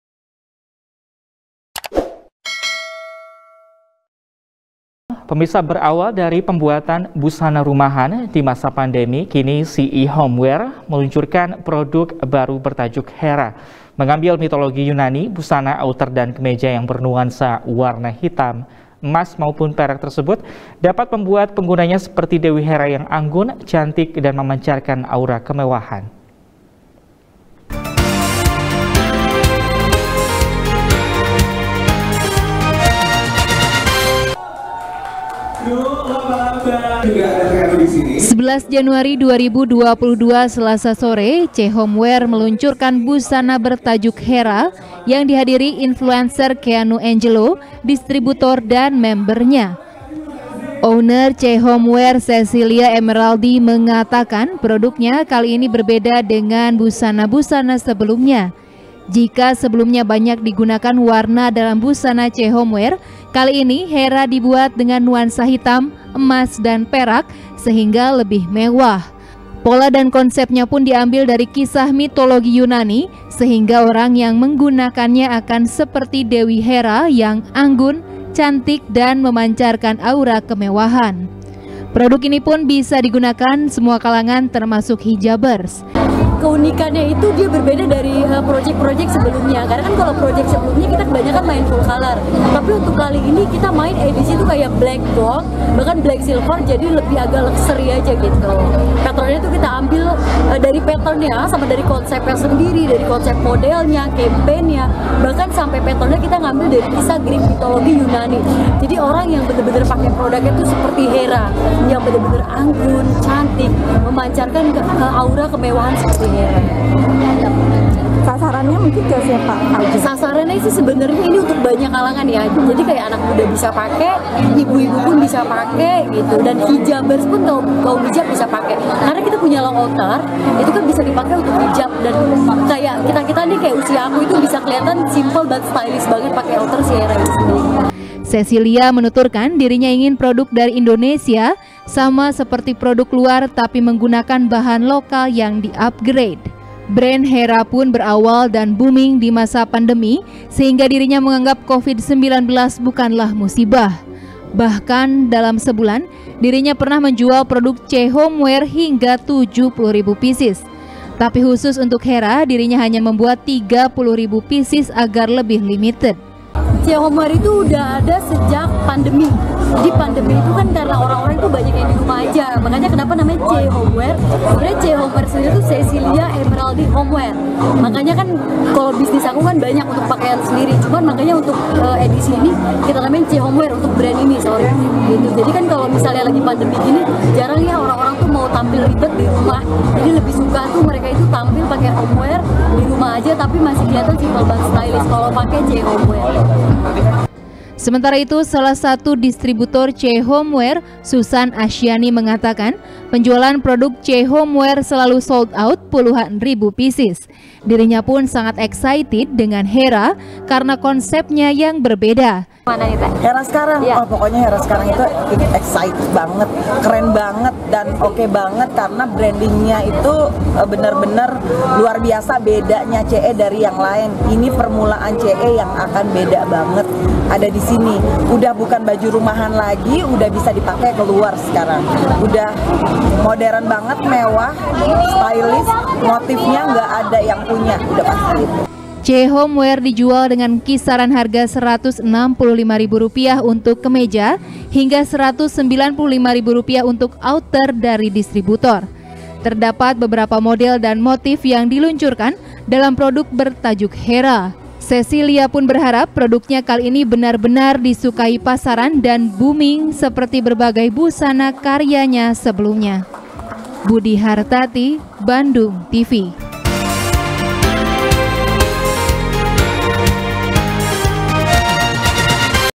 Bergaya Elegan Dengan Busana “Hera” Dari Ce Homewear | SBR | BANDUNGTV
SBR | BANDUNGTV | IRFAN RAMANDA | Berawal Dari Pembuatan Busana Rumahan Di Masa Pandemi, Kini Ce Homewear Meluncurkan Produk Baru Bertajuk Hera. Mengambil Mitologi Yunani, Busana Outer Dan Kemeja Yang Bernuansa Warna Hitam Emas Maupun Perak Tersebut, Dapat Membuat Penggunanya Seperti Dewi Hera Yang Anggun, Cantik, Dan Memancarkan Aura Kemewahan.